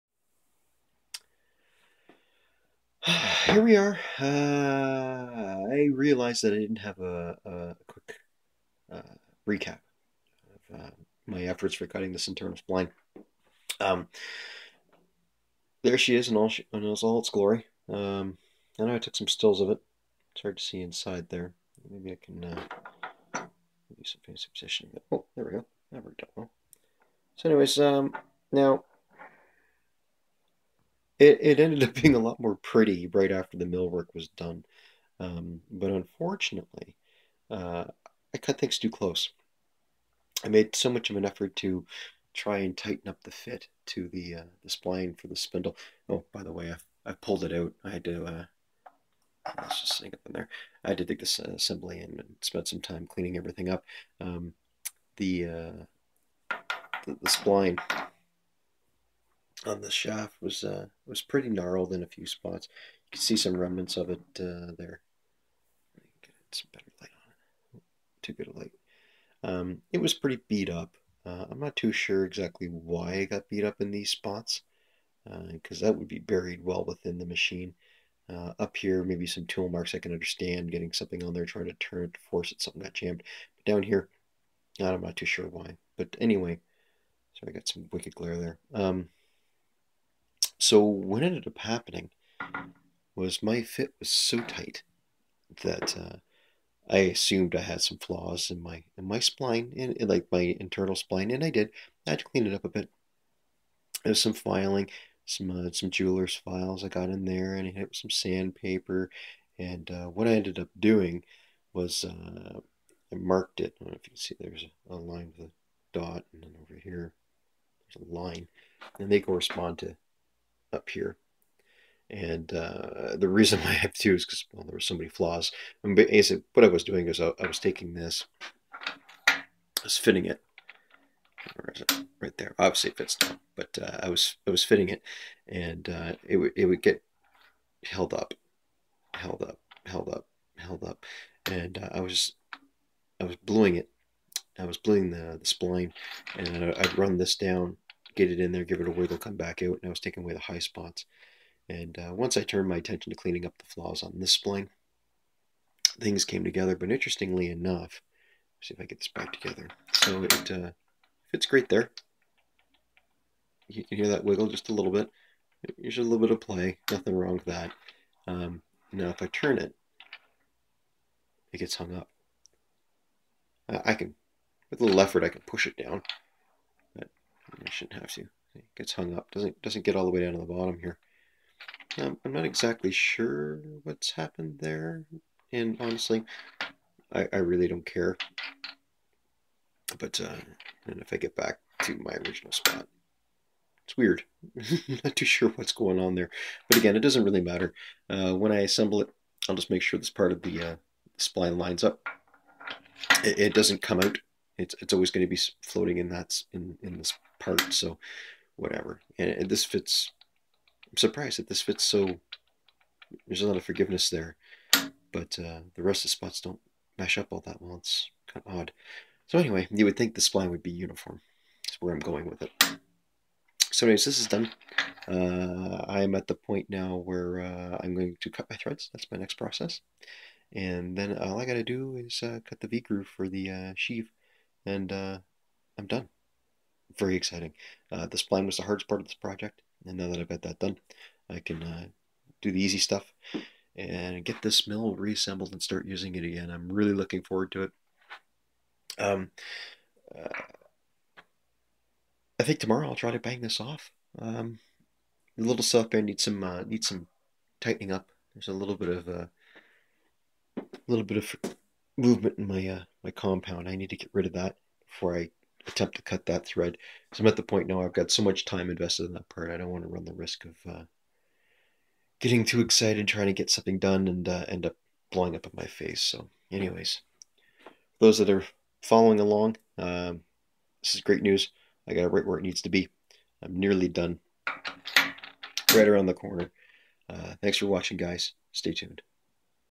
Rockwell 21-100 Spindle Hub episode #4
Quick recap of the results from my internal spline cutting efforts for the drive hub project.